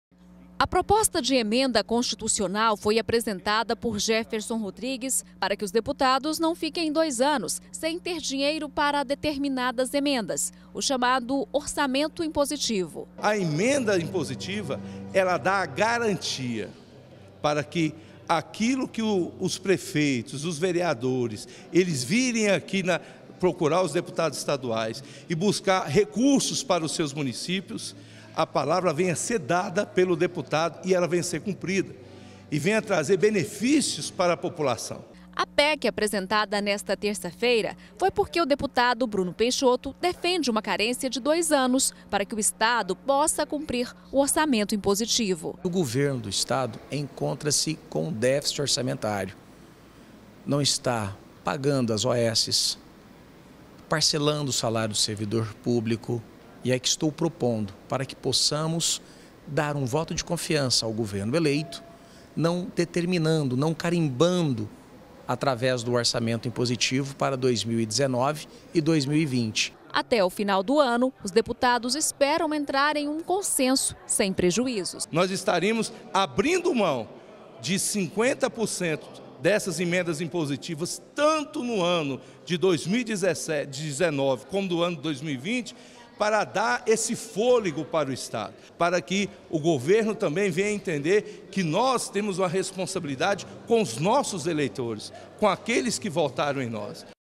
Orçamento impositivo: deputado quer manter verbas para 2019 e 2020
Foi apresentada hoje, na assembleia legislativa, uma proposta de emenda constitucional pra evitar que falte dinheiro para emendas parlamentares nos próximos dois anos. A proposta de congelamento é defendida pelo MDB, para dar um fôlego para o novo governo, que deve pegar o estado endividado.